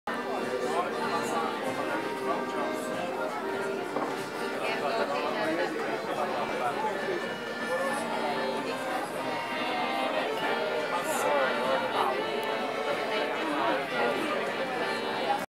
Napoli piazza Bellini
Al tavolino di uno dei caffè letterari di piazza Bellini a Napoli.